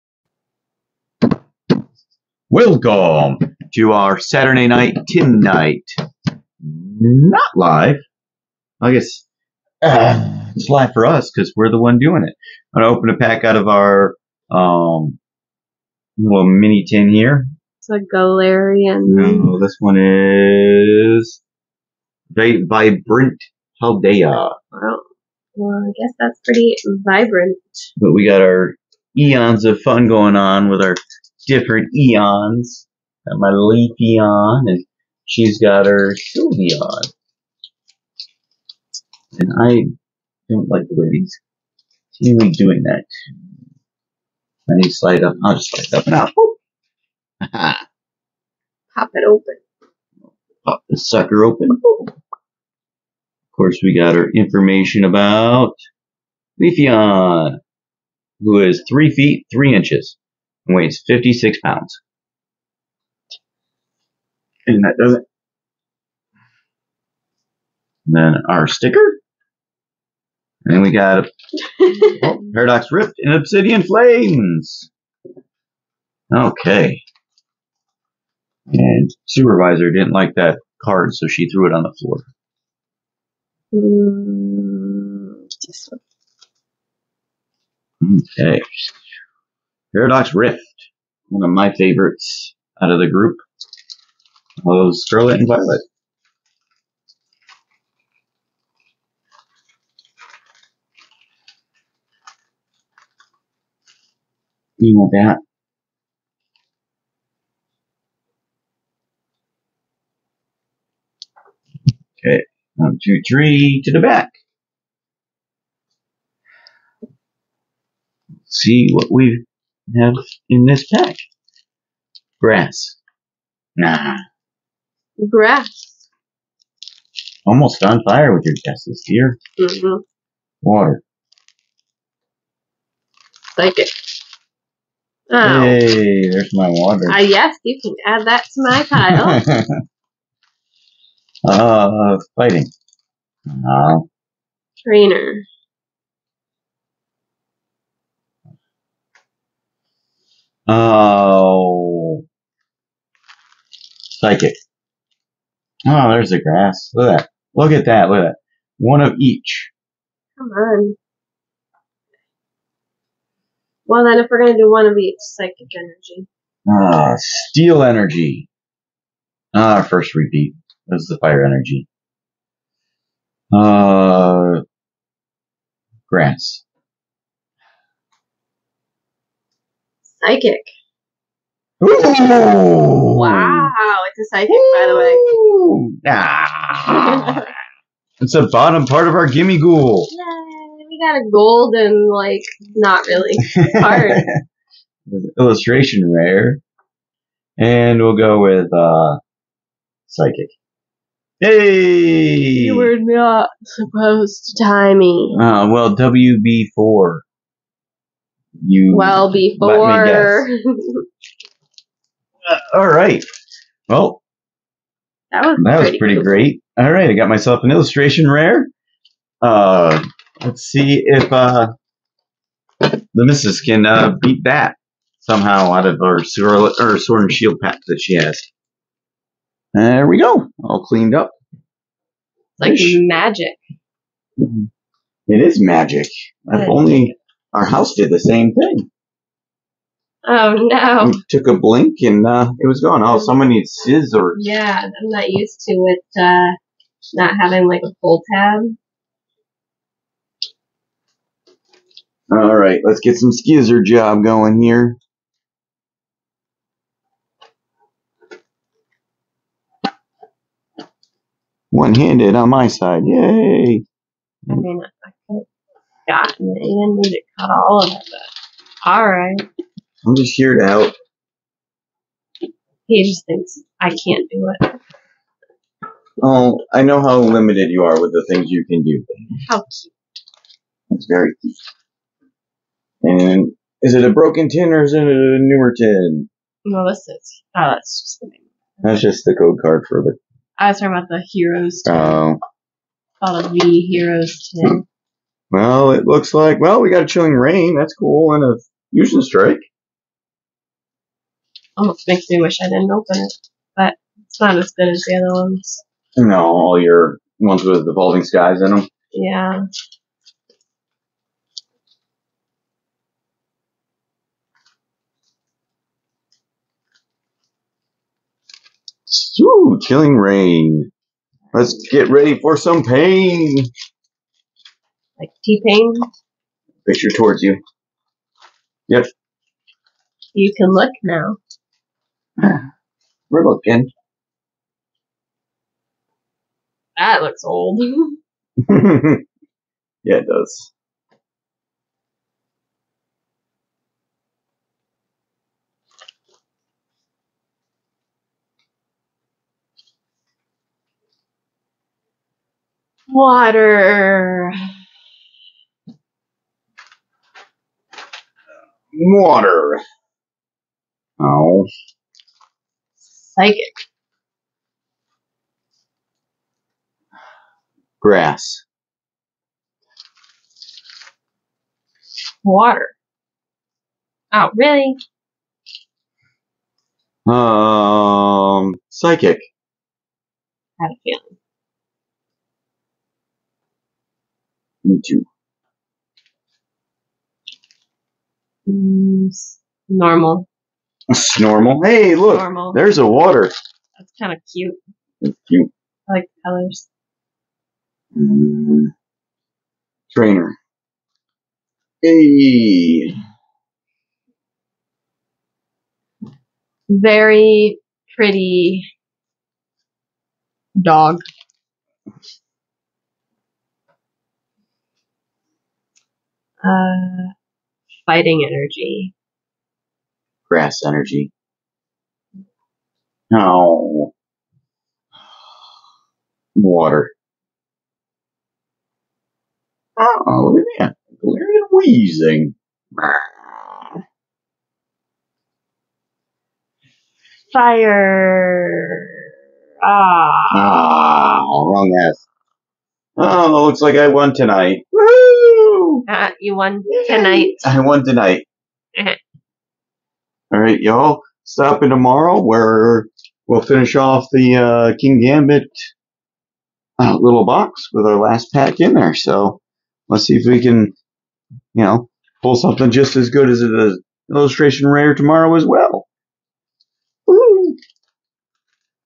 Which Eon Has the better HIT??
Happy Saturday Night Tin Night Not Live!! Who's tin is going to win tonight's pack battle? Will my leafeon came victory or Will my wife's Sylveon take it from me?

Would you like a Heavy Hitters Premium collection box yourself?